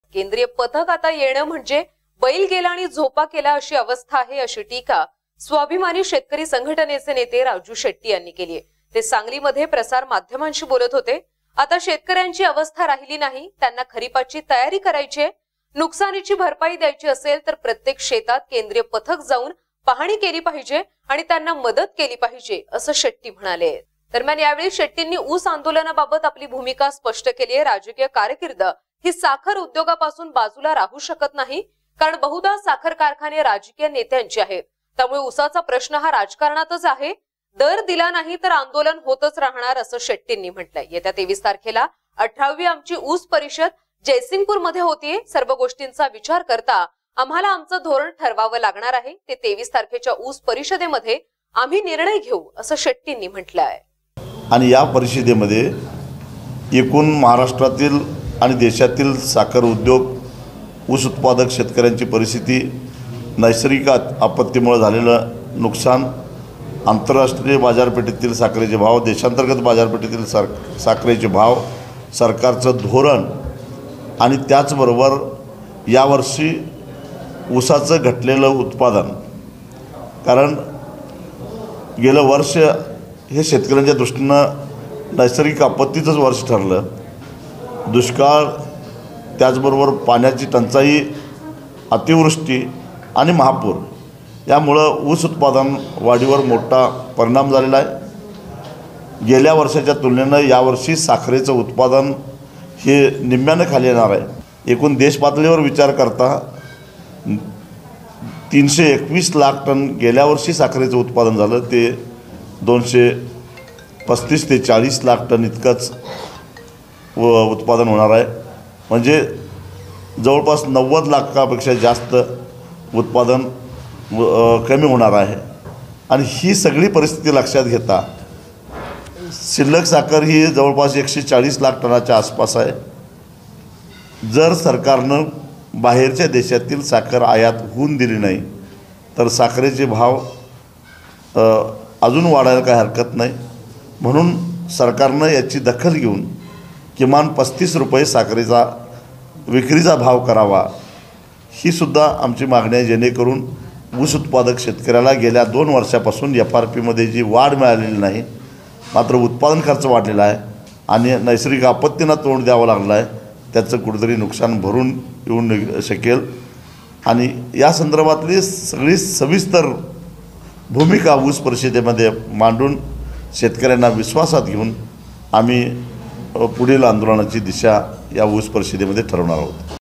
RAJU SHETTY CRITICIZE CENTRAL GOVERNMENT OF INDIA
CRITICIZE CENTRAL GOVERNMENT OF INDIA

केंद्रीय पथक आता येणे म्हणजे बैल गेला आणि झोपा केला अशी अवस्था आहे.. अशी टीका स्वाभिमानी शेतकरी संघटनेचे नेते राजू शेट्टी यांनी केली आहे.. ते सांगली मध्ये प्रसार माध्यमांशी बोलत होते.पहा काय म्हणाले राजू शेट्टी , फक्त MKN मराठी वर